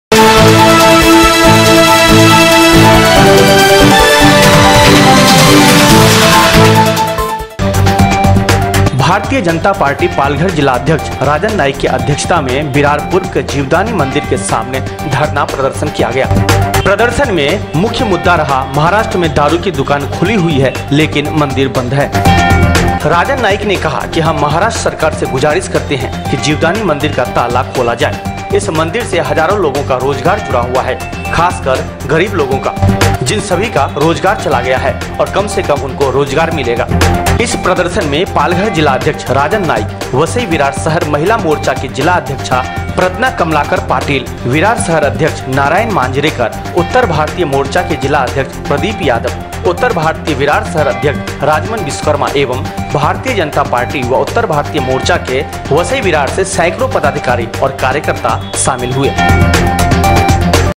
जिवदानी मंदीर खोलने के लिए भारतीय जनता पार्टी का धरना प्रदर्शन India Samachar Mumbai
जिवदानी मंदीर खोलने के लिए भारतीय जनता पार्टी का धरना प्रदर्शन
India Samachar Mumbai